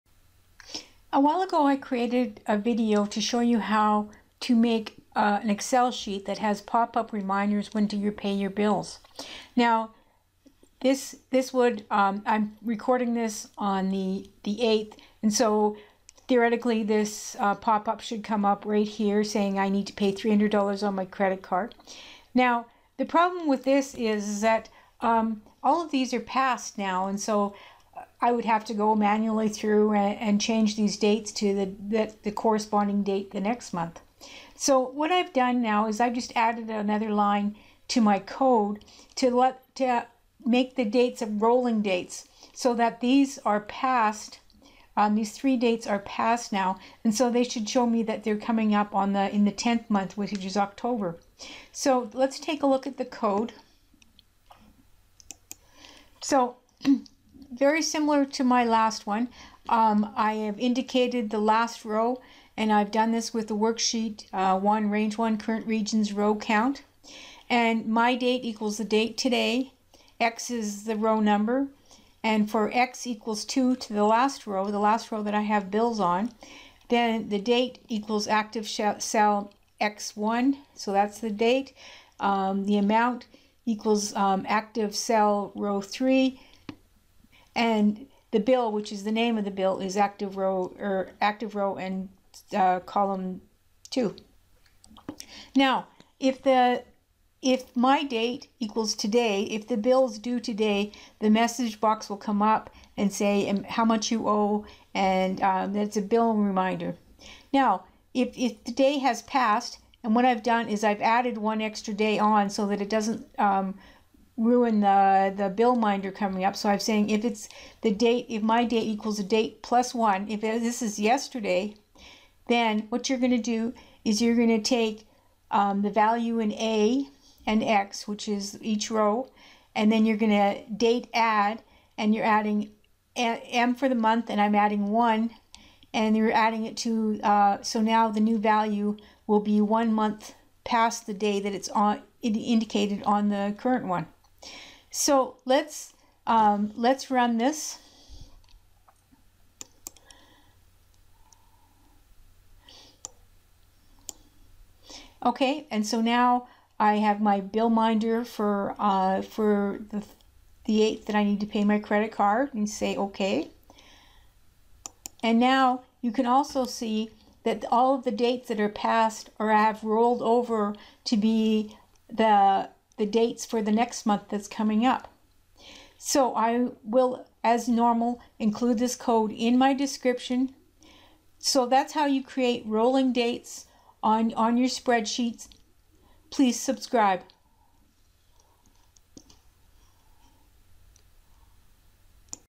How to create dates that change automatically when they expire with VBA
How to create rolling dates on your sheet in Excel.  VBA code included.  When the date has passed it will change to your date for next month. Dates will roll over to the next month automatically when they expire.
Code
Private Sub Workbook_Open()
Dim x As Integer
Dim lastrow As Long

lastrow = Worksheets("sheet1").Range("A1").CurrentRegion.Rows.Count

mydate = Date
x = 2
For x = 2 To lastrow

Bill = ActiveSheet.Cells(x, 2).Value

thedate = ActiveSheet.Cells(x, 1).Value
Amount = ActiveSheet.Cells(x, 3).Value

If mydate = thedate Then
    MsgBox Bill & vbCrLf & "$" & Amount, 0, "Bill Reminder"
End If
If mydate not equal to  thedate + 1 Then

   Range("A" & x).Value = DateAdd("m", 1, Range("A" & x).Value)
 End If

Next
End Sub

easyexcelanswers